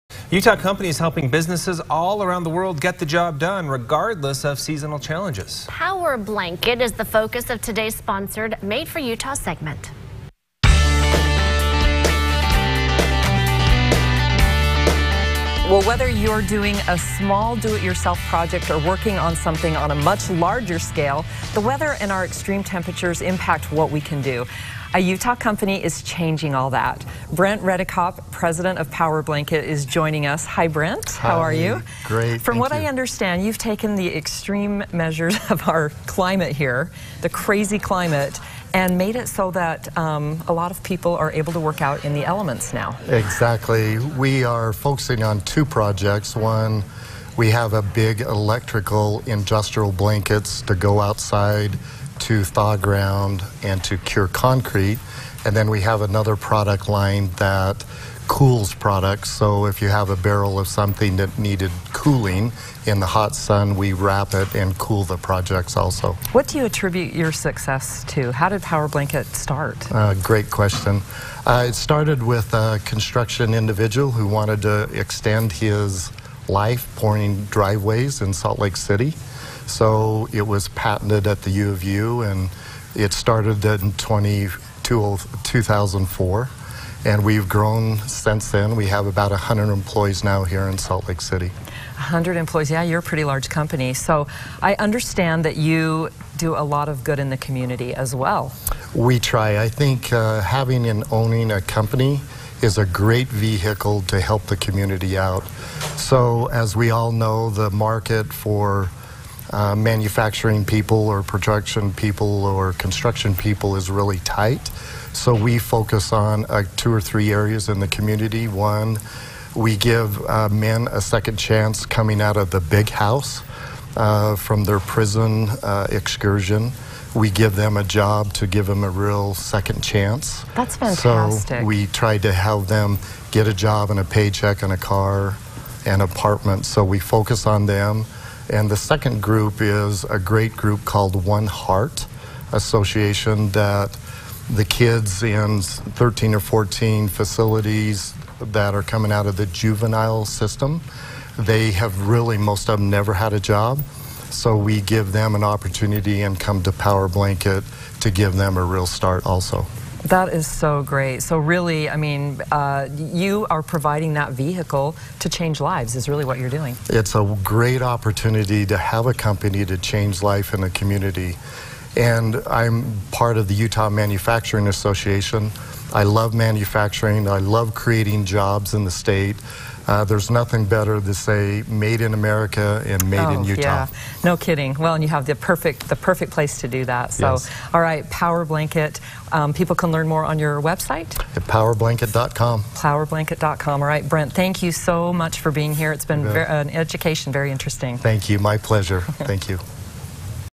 Made4Utah: Powerblanket
Powerblanket helps companies extend their construction and production window despite extreme climate conditions.